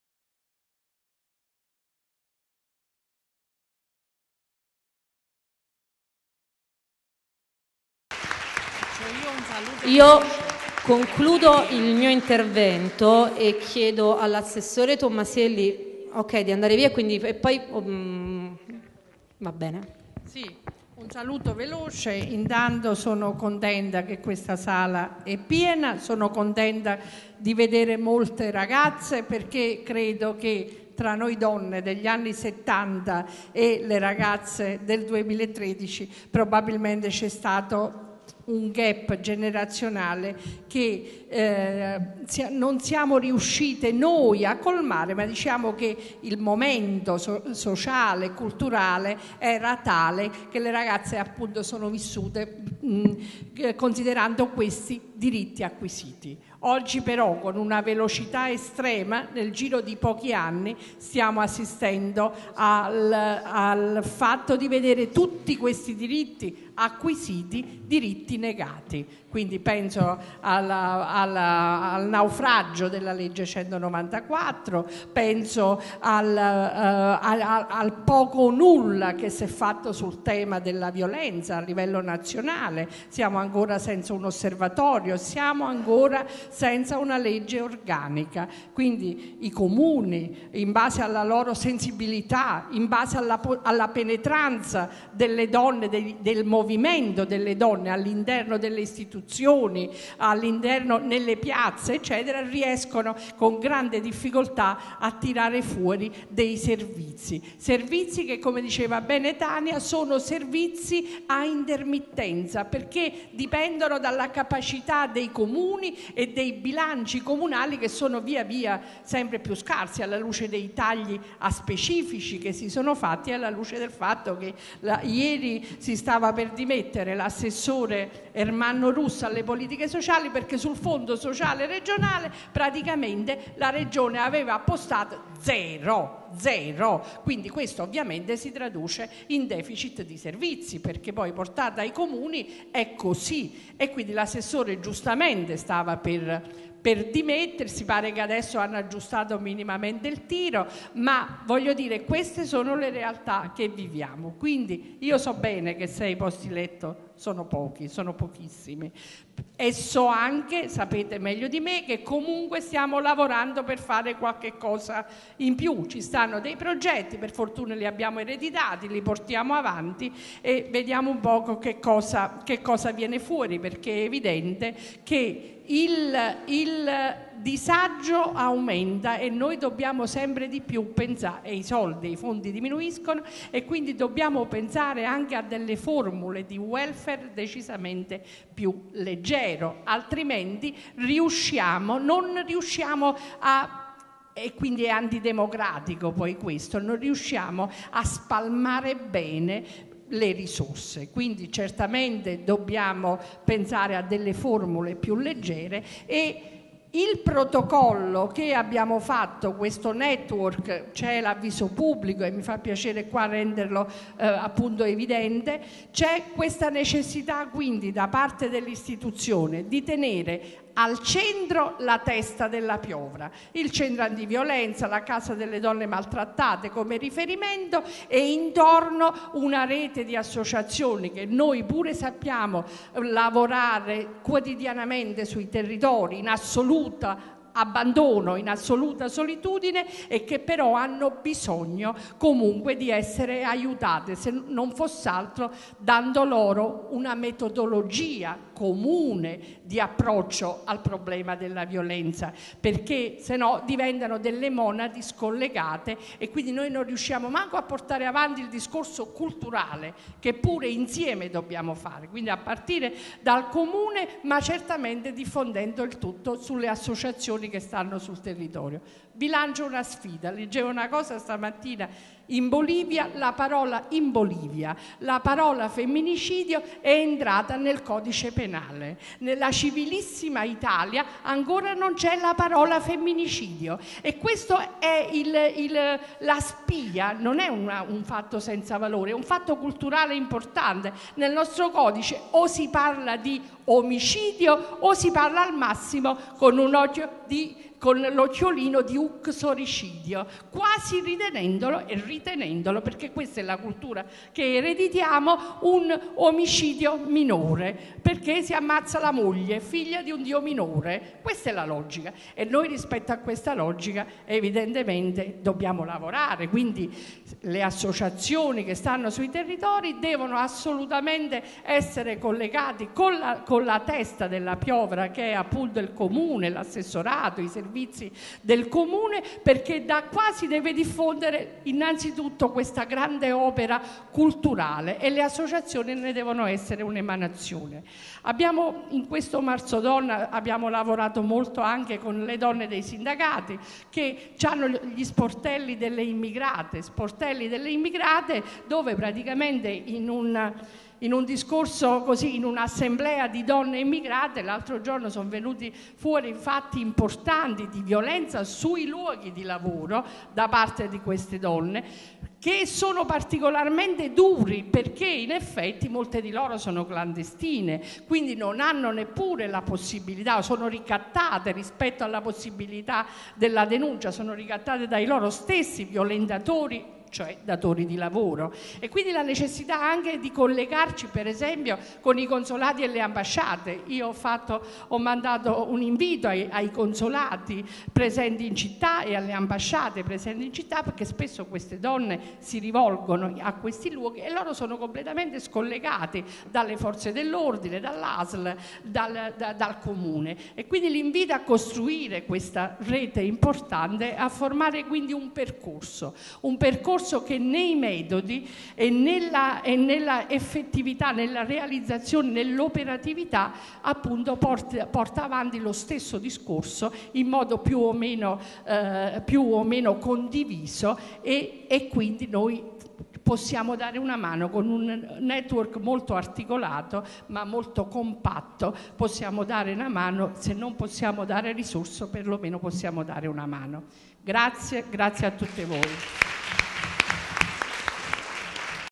Giuseppina Tommasielli - Non solo una casa! Il ruolo strategico dei servizi per donne maltrattate
NON SOLO UNA CASA!
IL RUOLO STRATEGICO DEI SERVIZI PER DONNE MALTRATTATE
Mercoledì 20 Marzo 2013
Antisala dei Baroni - Maschio Angioino Napoli
Per info: